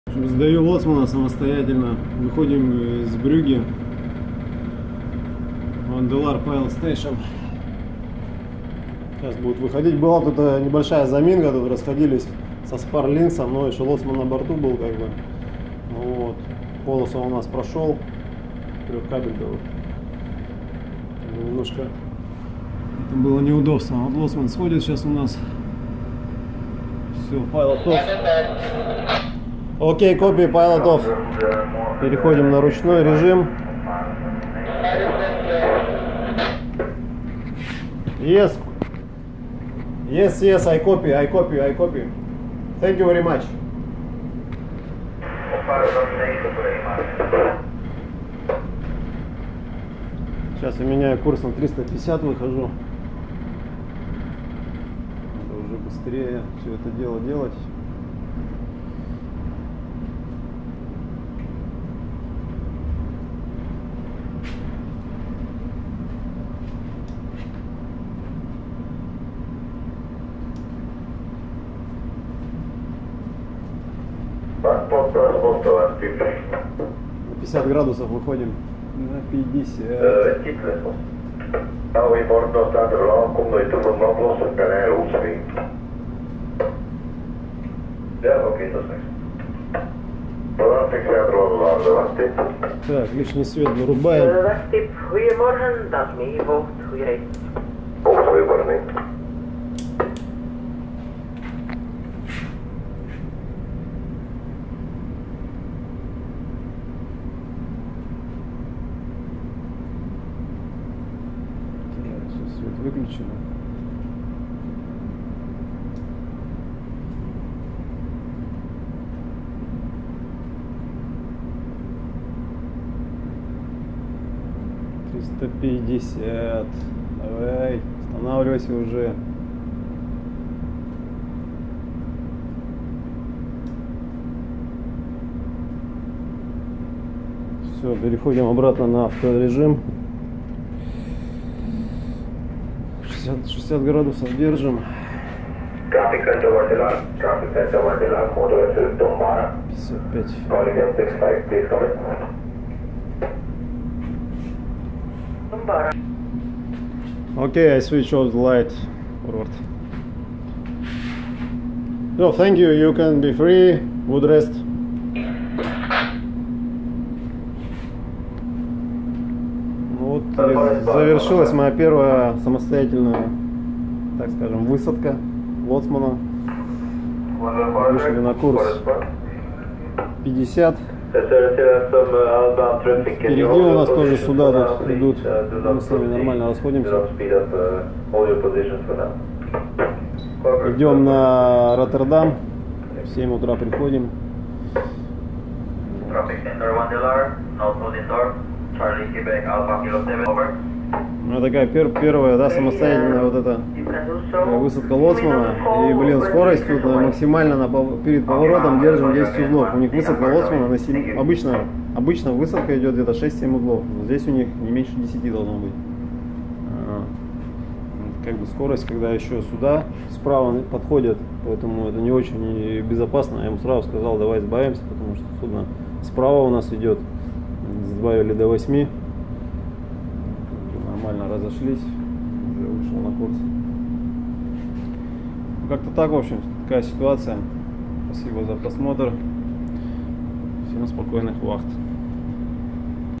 Сдаю лоцмана
Впервые самостоятельно, без капитана сдаю лоцмана. Хорошо когда кэпы с железными нервами и доверяют ответственную работу, с другой стороны, а надо ли оно если ты ниже в должности на 2 пункта? Но тем не менее приказ есть приказ.